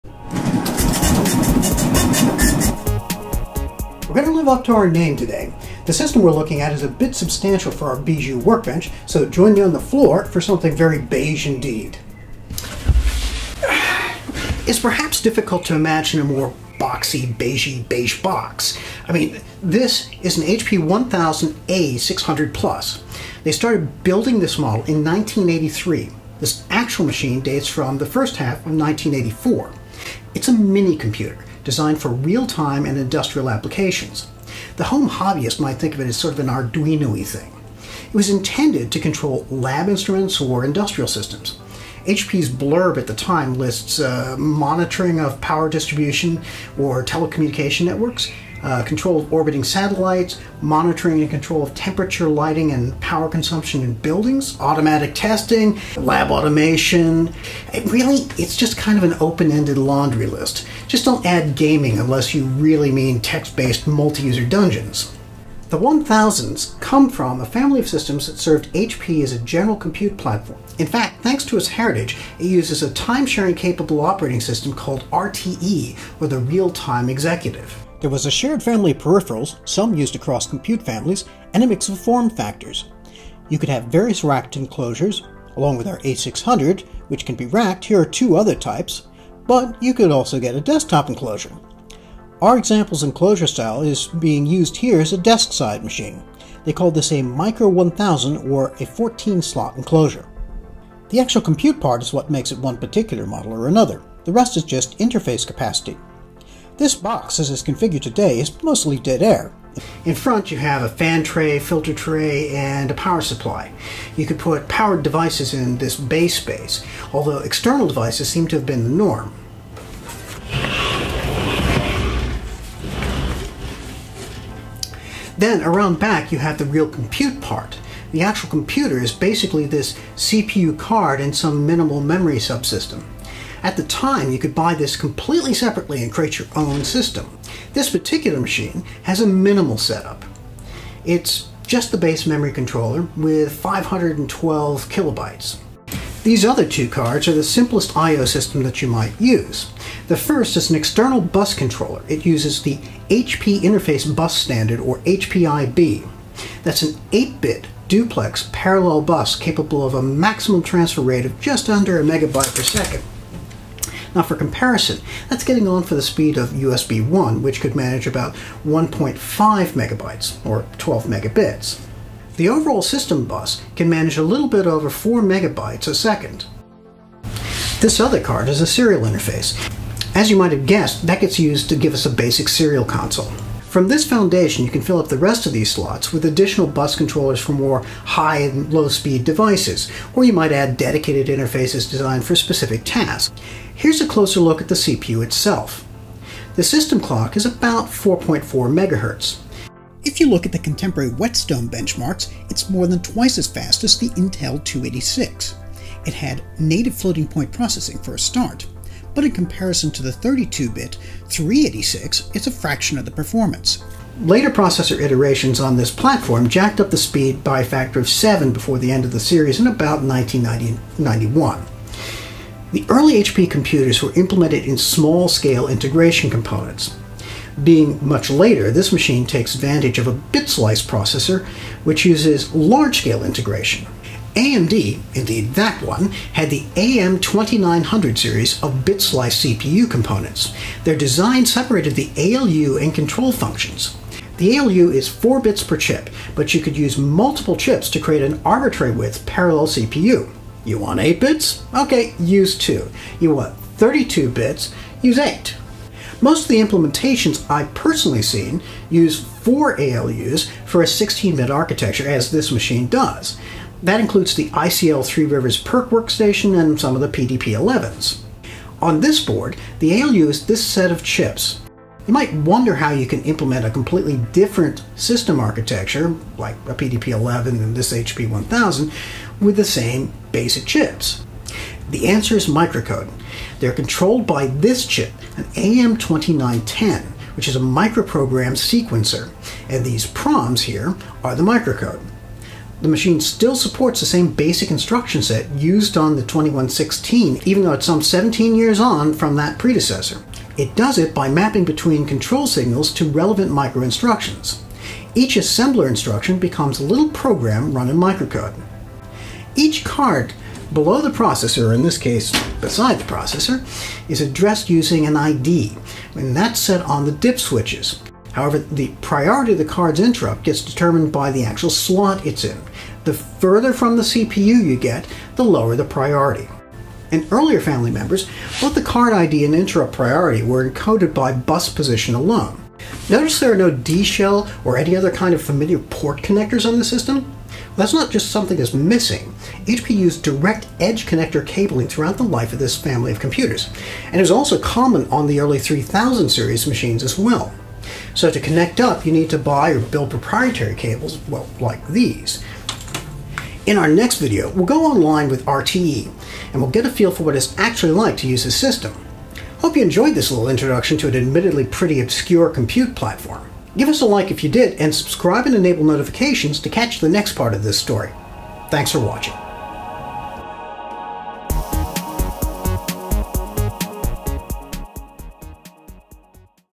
HP 1000 A600+ Vintage Minicomputer Introduction
This is an introduction to the HP 1000 A-Series computers starting with one of the last of the series -- the A600+.  It’s a 16-bit minicomputer from a long live family.  We’ll have a quick look at the system and how it’s put together and what it’s for.

Photo Credits:
- 140-foot Control Room Upgrade by greenbankobservatory is licensed underCC BY-NC-ND 2.0

HP performance data:
- RTE-A Performance Brief A ~Series Automaters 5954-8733 June 1985
- MIPS - HP 1000 A-Series Computer Handbook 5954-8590 Dec 1989